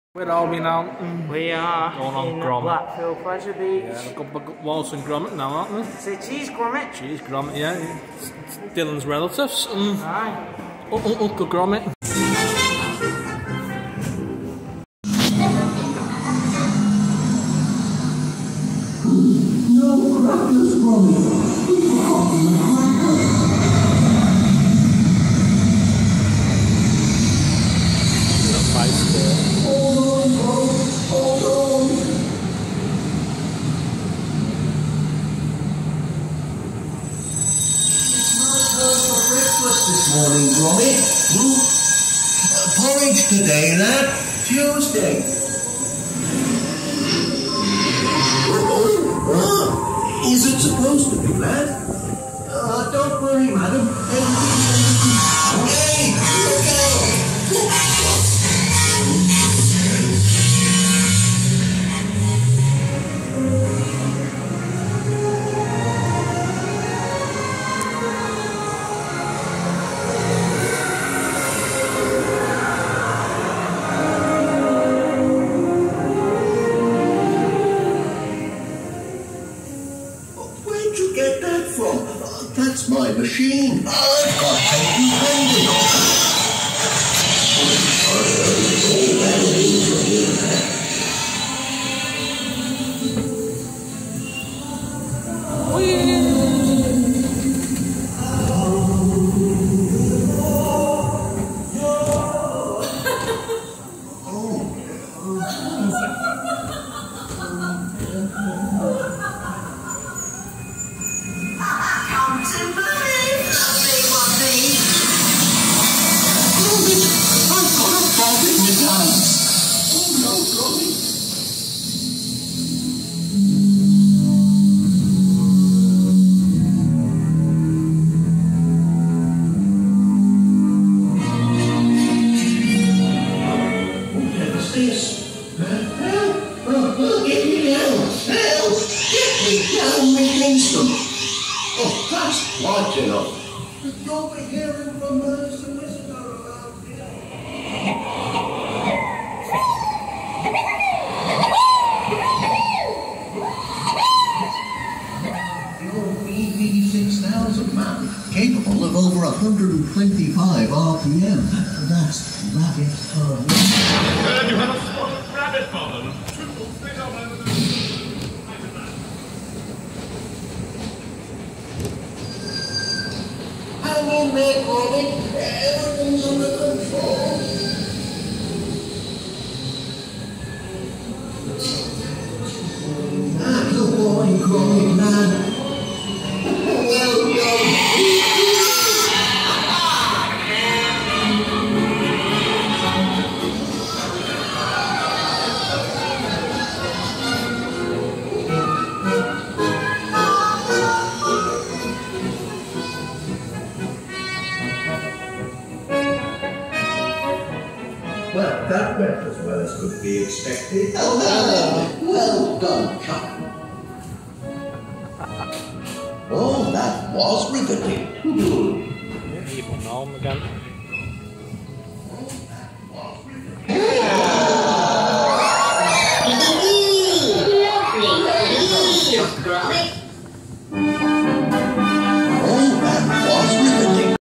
Wallace and gromit ride Blackpool pleasure beach September 2021
we always love going on the Wallace and gromit ride at Blackpool pleasure beach